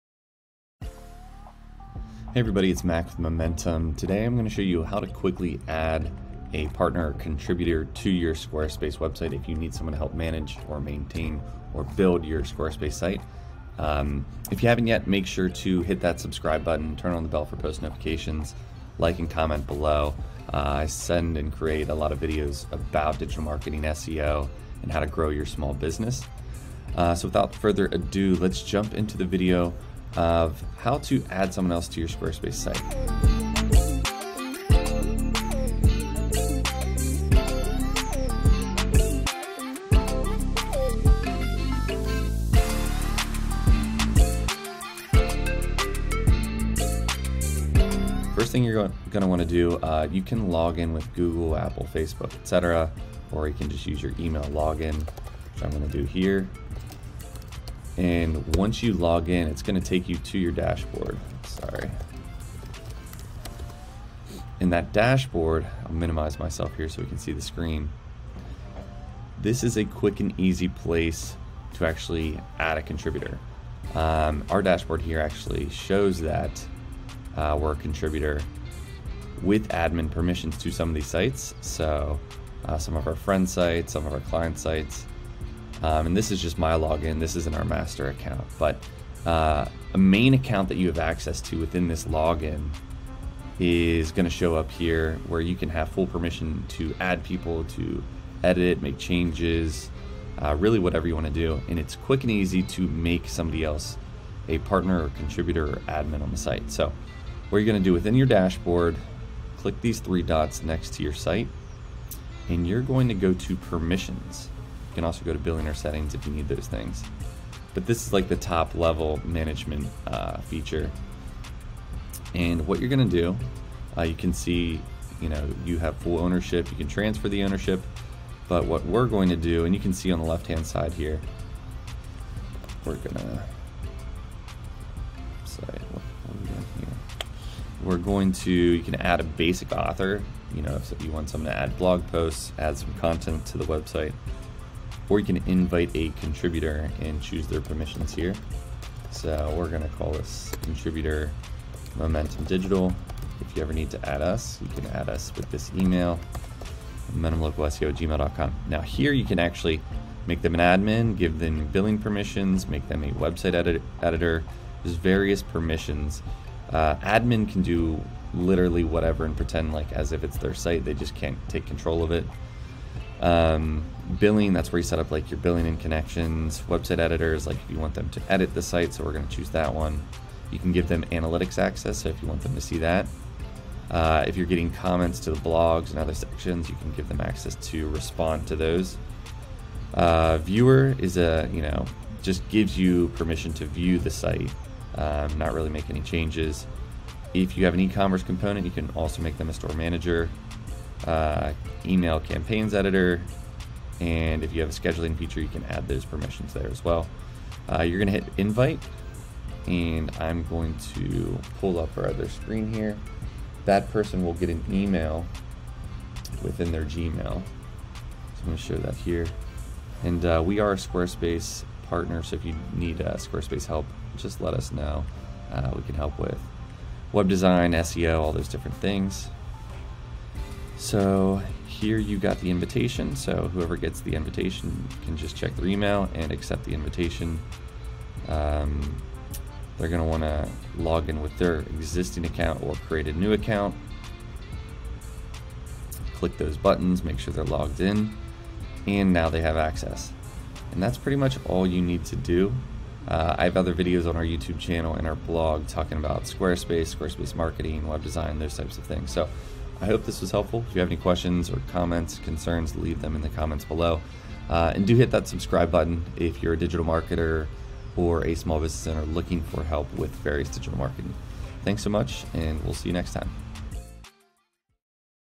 How to Add a User to Squarespace
Learn How to Add a User, Contributor, Partner or Manager to Your Squarespace Website in a few easy steps.

This is helpful to learn if you are the business owner or marketer and need to partner or hire with someone to help edit and manage your Squarespace site.

Here are the steps to follow for success.

Steps
1. Login to your account
2. Go to your Dashboard
3. Click into settings for the proper account (3 dots)
4. Click on "Permissions"
5. Click "Invite Contributor"
6. Add their name and email
7. Choose their permission level
-- Check the boxes for permission level you want to give
-- Choose this based on what access level you want them to have
8. Add and save
9. They can accept the email invite on their end
10. They might need to create an account if they dont have one